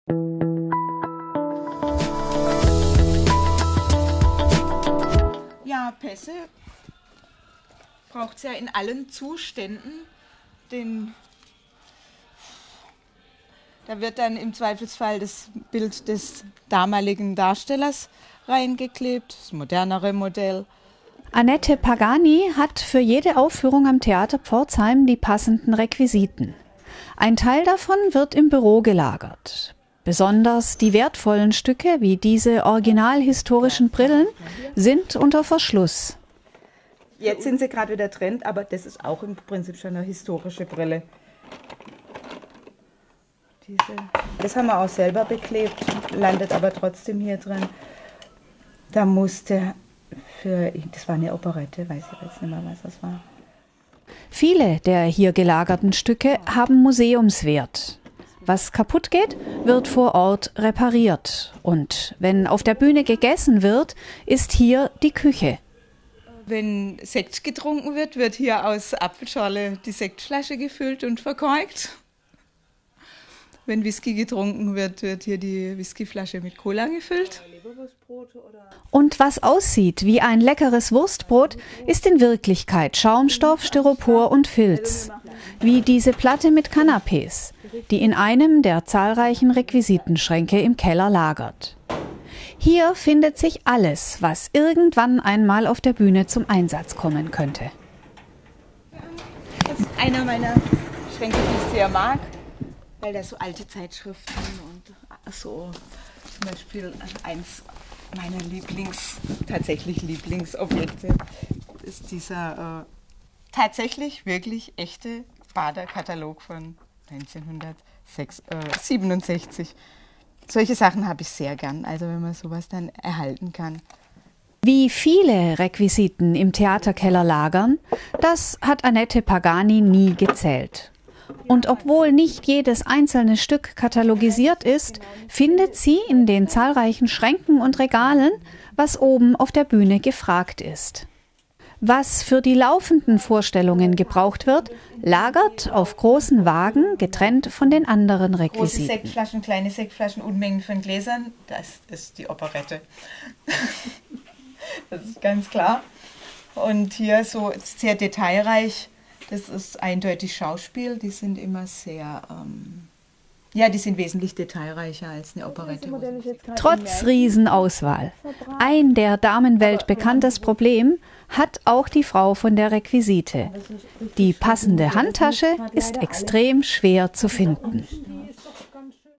Falsches Wurstbrot und historische Brillen
Ein Salamibrot aus Filz und Schaumstoff, kistenweise Brillen, Regale voller Telefone. In der Requisite am Theater Pforzheim findet sich für jede Aufführung das passende Zubehör.